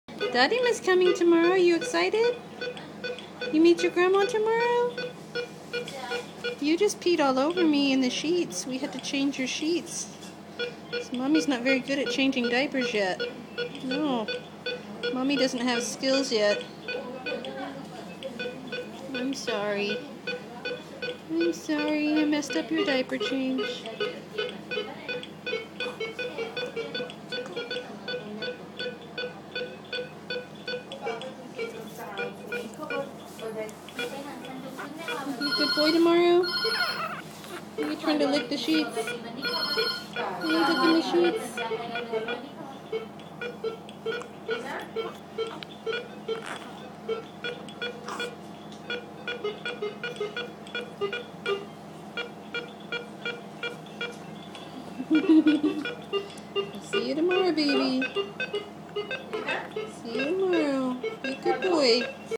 Vivek peed on sheets
Giving my four pound son a stern talking to for peeing on the sheets in his isolette in the Anand NICU.